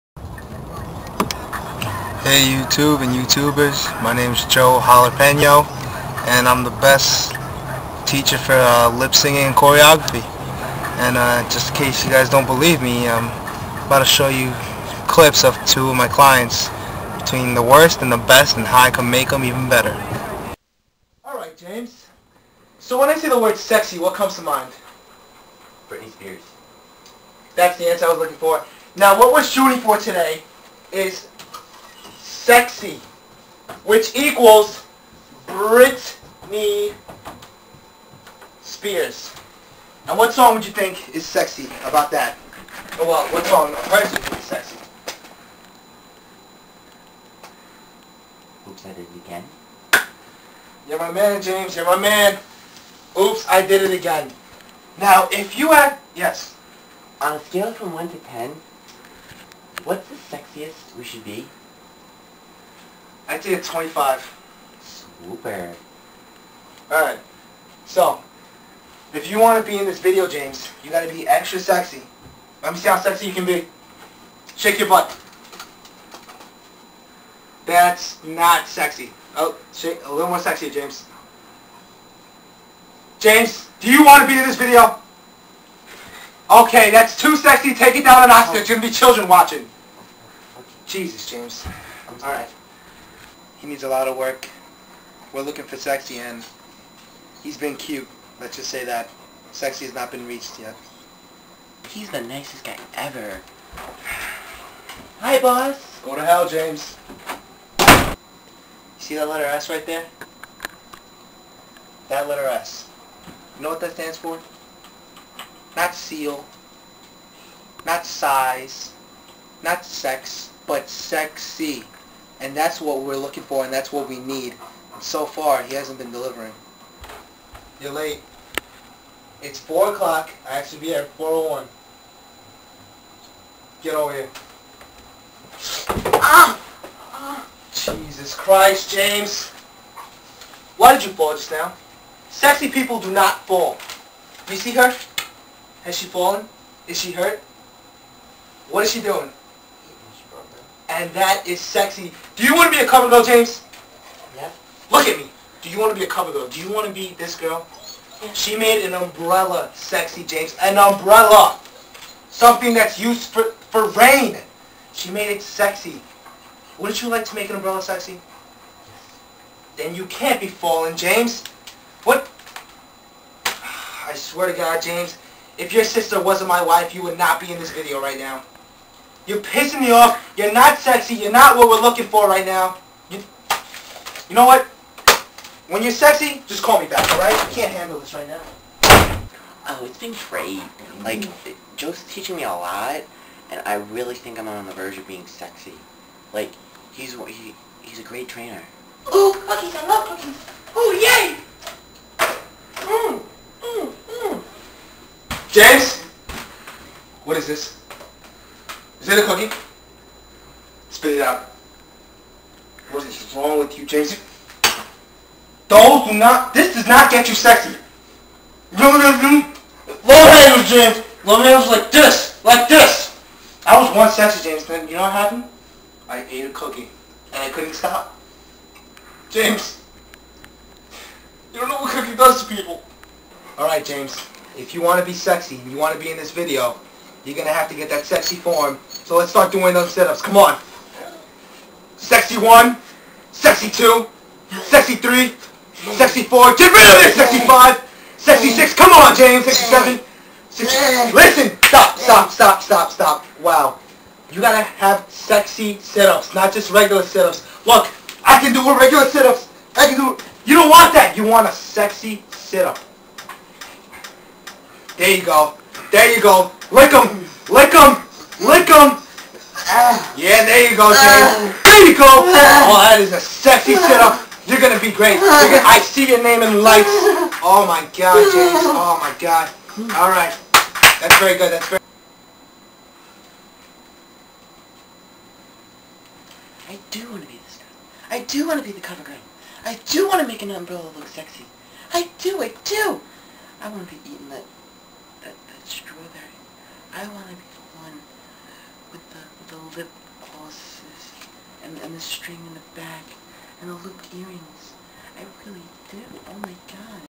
The Joe Jalapeño Story (Part 1 of 6)
The first part to a 6 part documentary about the imfamous lip sync coach, Joe Jalapeno and how he taught two young hopefulls the skills they need to be sucessful in the lip syncing world.